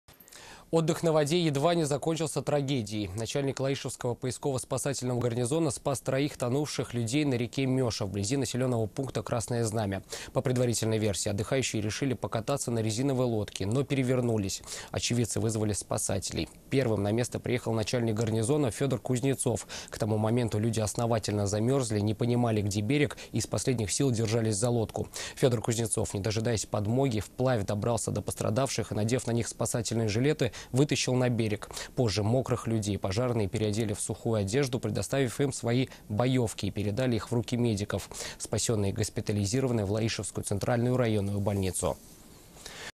Отдых на воде едва не закончился трагедией - ТНВ
Отдых на воде едва не закончился трагедией.  Начальник Лаишевского поискового-спасательного горнизона спас троих тонувших людей на реке МЁша, вблизи населенного пункта Красное Знамя...
По предварительной версии, отдыхающие решили покататься на резиновой лодке, но перевернулись. Очевидцы вызвали спасателей. Первым на место приехал начальник гарнизона - Федор Кузнецов. К тому моменту люди основательно замерзли, не понимали где берег и из последних сил держались за лодку. Фёдор Кузнецов не дожидаясь подмоги, вплавь добрался до пострадавших и, надев на них спасательные жилеты, вытащил на берег. Позже мокрых людей пожарные переодели в сухую одежду, предоставив им свои "боёвки" и передали их в руки медиков. Спасённые госпитализированы в Лаишевскую Центральную районную больницу.

Координаты:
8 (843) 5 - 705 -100